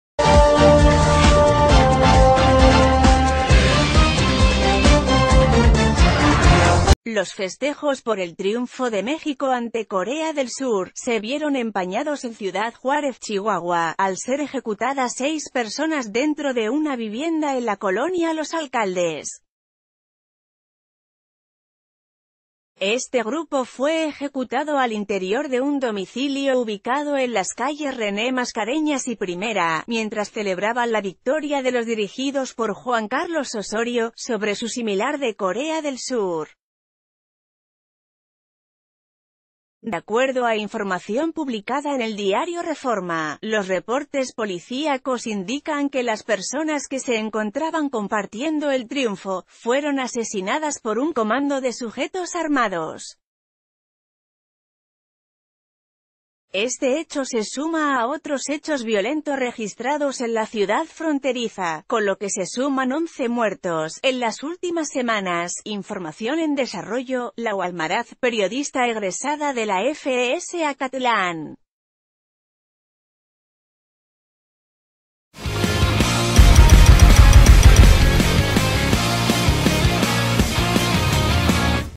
Asesinan a seis personas durante festejo de la Selección Mexicana ante Corea del Sur
Los festejos por el triunfo de México ante Corea del Sur, se vieron empañados en Ciudad Juárez, Chihuahua, al ser ejecutadas seis personas dentro de una vivienda en la colonia Los Alcaldes. Este grupo fue ejecutado al interior de un domicilio ubicado en las calles René Mascareñas y Primera, mientras...